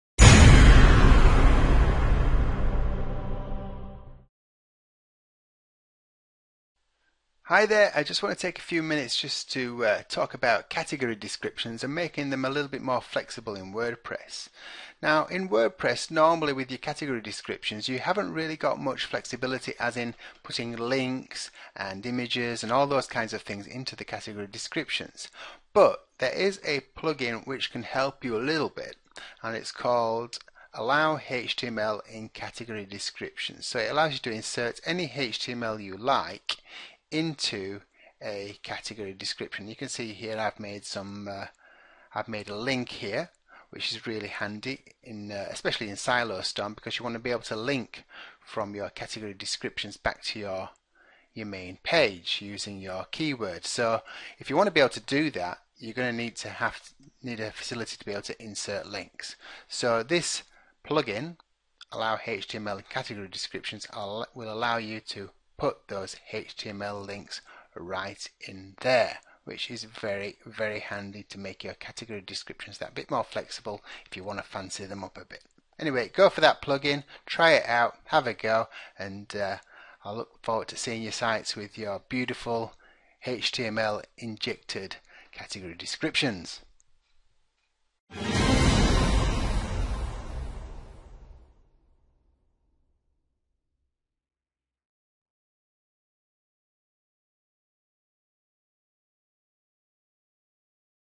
Tip for HTML in Category Descriptions in WordPress
This is a quick tip for WordPress users who need HTML in category descriptions. Its particularly useful for users of Ultimate Silo Storm Pro WordPress Theme when building silo based websites. The plugin mentioned can be found here...